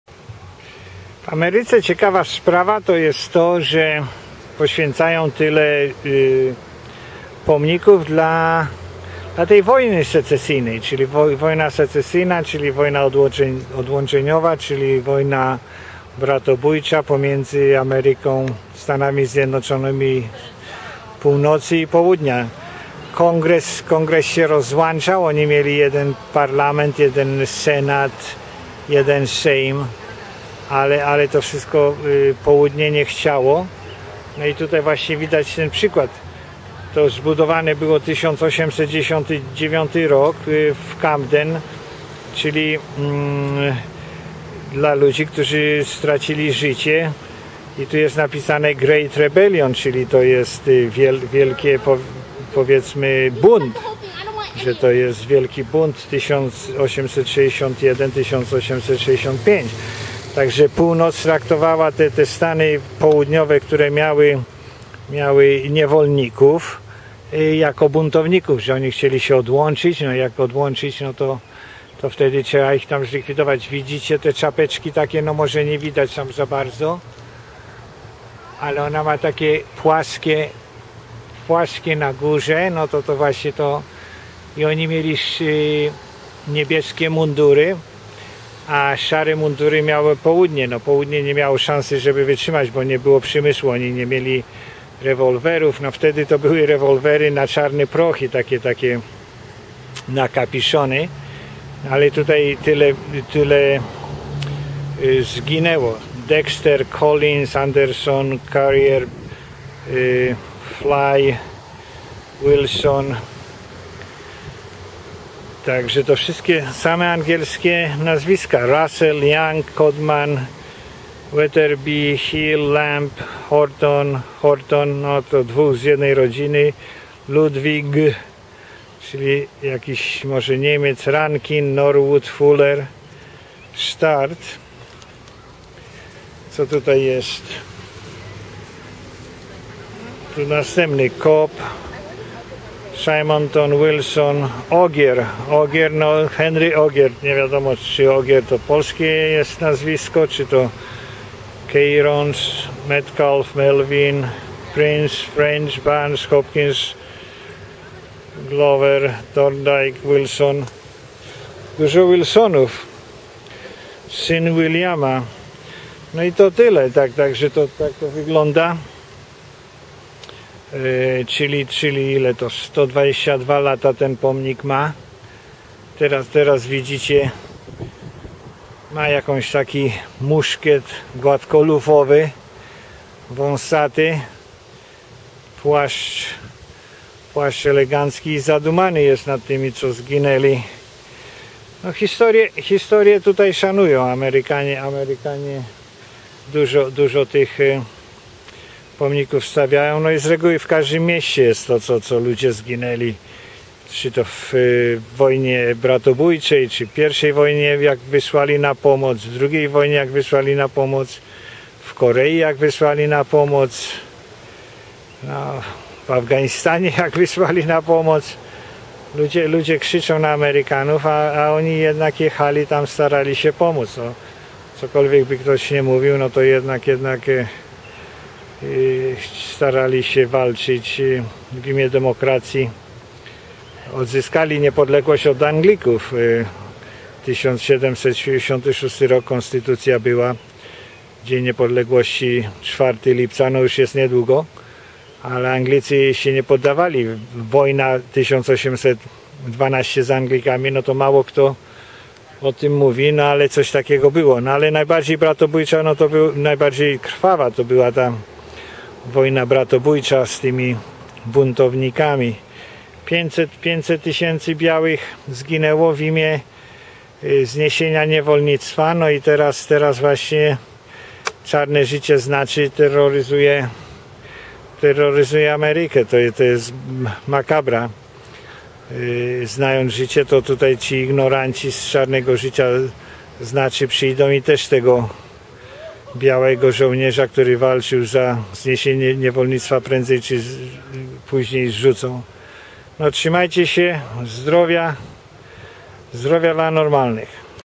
Pomnik Poświęcony Zołnierzom Armii Północnej Zabitym W Walce Z Południowymi Buntownikami
Południe odłączyło się od reszty stanów zjednoczonych to północ najechała stany SECESYJNE =ODŁĄCZENIOWE